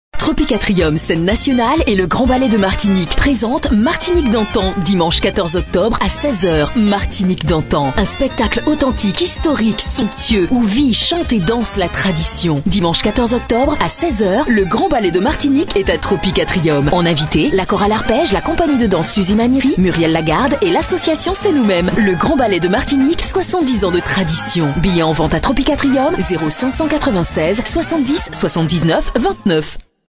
Bande-annonce Grand Ballet de Martinique 2018
Le dimanche 14 octobre, retrouvez le Grand Ballet de Martinique à Tropiques Atrium !!!

Depuis 1946, le Grand Ballet de Martinique est l’un des plus beaux ballets de la Caraïbe avec une trentaine de musiciens, danseurs et danseuses qui s’érigent en ambassadeurs des chansons et des chorégraphies de la Martinique.